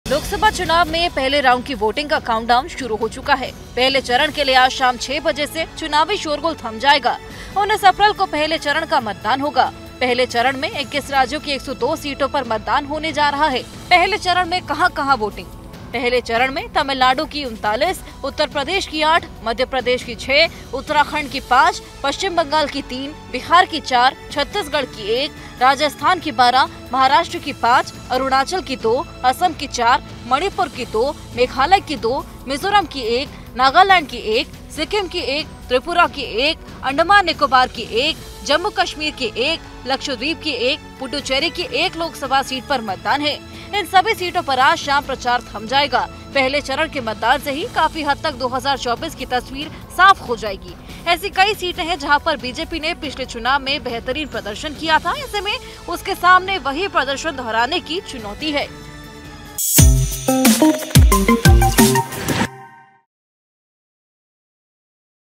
Lok Sabha Election 2024: पहले चरण में 19 अप्रैल को जानें किस राज्य की कितनी सीटों पर होगा मतदान?
पहले चरण के लिए आज शाम 6 बजे से चुनावी शोर थम जाएगा...19 अप्रैल को पहले चरण का मतदान होगा...पहले चरण में 21 राज्यों की 102 सीटों पर मतदान होने जा रहा है...